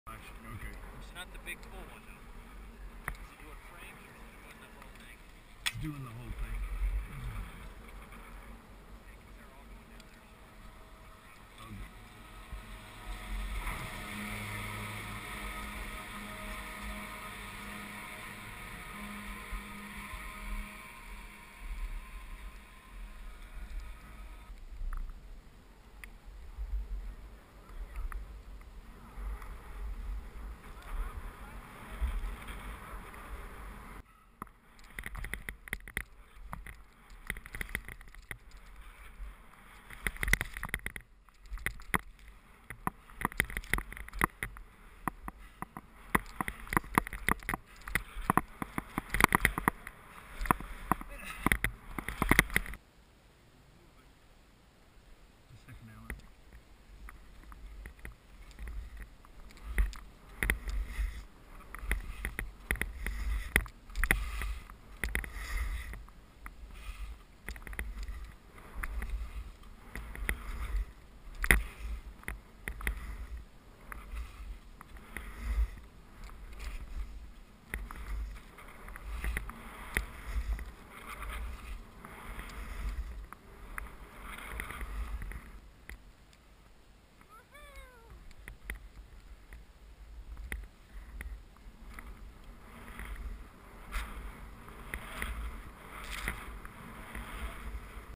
Day 4 - Aspen, Snowbird, Squaw Valley, and Mammoth Road Trip
Today is Day 4 (April 24, 2011) of the Spring Ski Road Trip to Aspen, Snowbird, Squaw Valley, and Mammoth Mountain. Bob Smith (Smitty) and Lance Shermoen continue skiing at Aspen Highlands with 8 inches of new powder. Today was closing day for all of Aspen Tomorrow we are driving to Snowbird